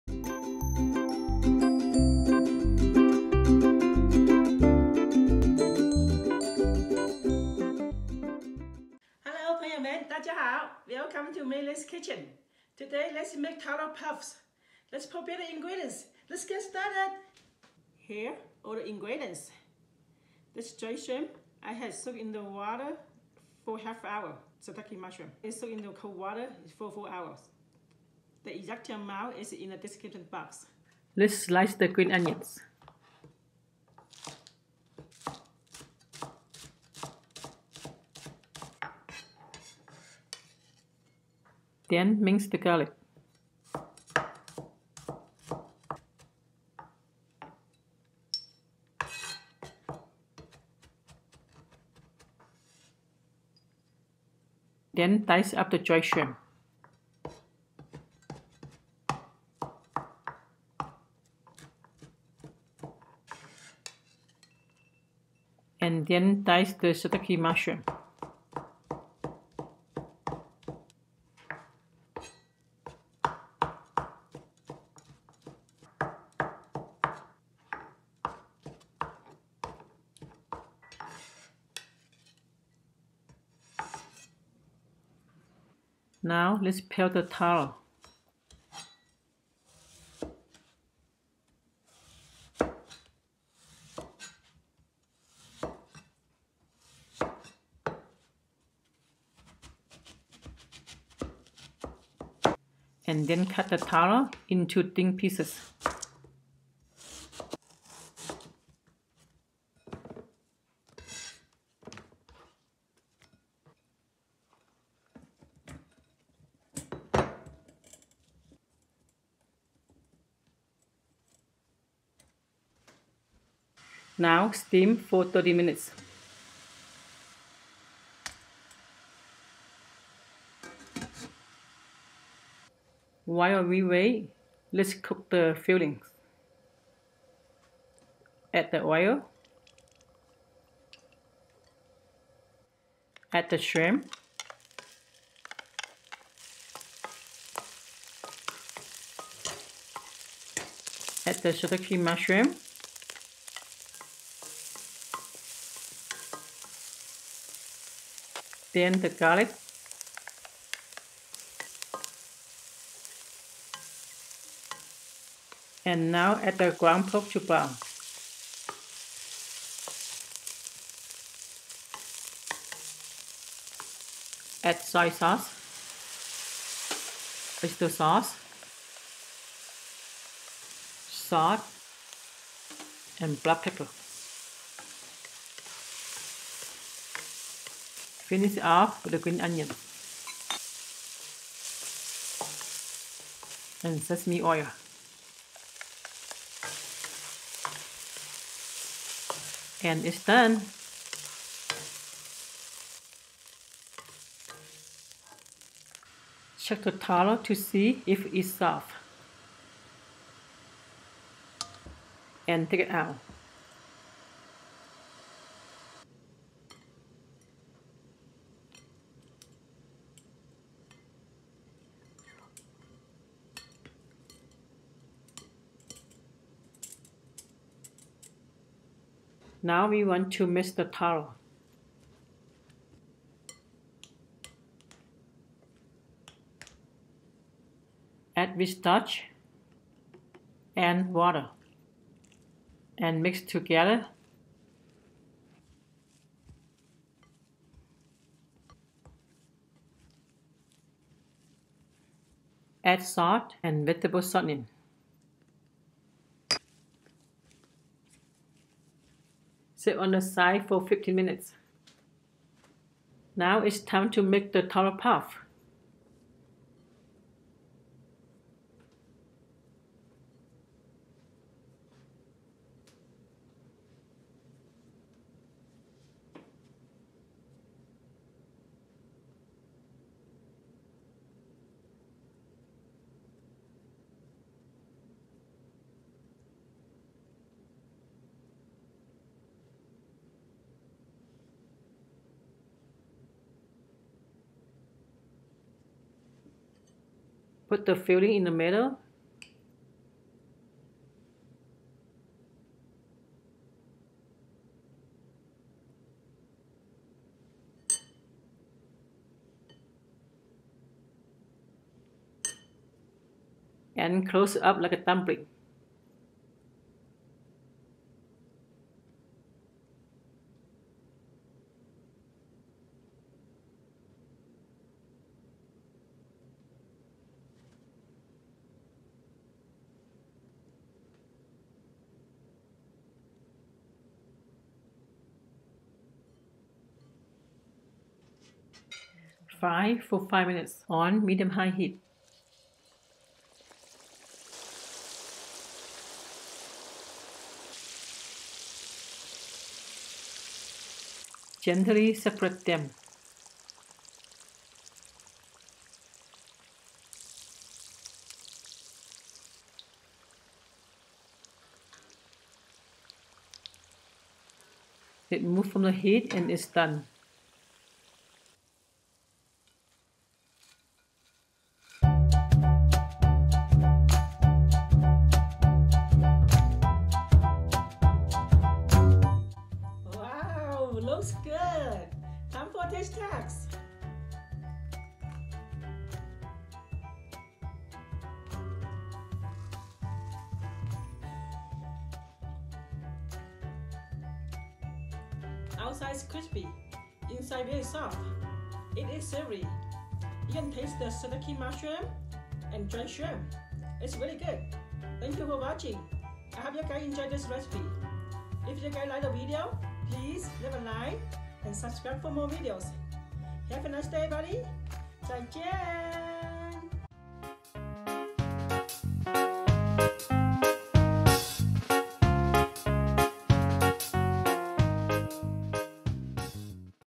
Homemade Yummy Dim Sum Recipe! Taro Puff Recipe!
Welcome to Mei Lian's kitchen! Today we have another Dim Sum recipe for you! We are going to be making taro puff. My family always gets this wonderful dish when we go out for dim sum. I hope you enjoy this recipe!

7 oz ground pork
2 cloves of garlic
1 green onion stalk
½ oz dried shrimp
1 ½ tsp soy sauce
1 ½ tsp oyster sauce
½ tsp sesame oil
¼ tsp salt
¼ tsp black pepper

Taro Puff Shell
1 lb taro
3 tbsp wheat starch
2 tbsp water
2 tsp vegetable shortening
½ tsp salt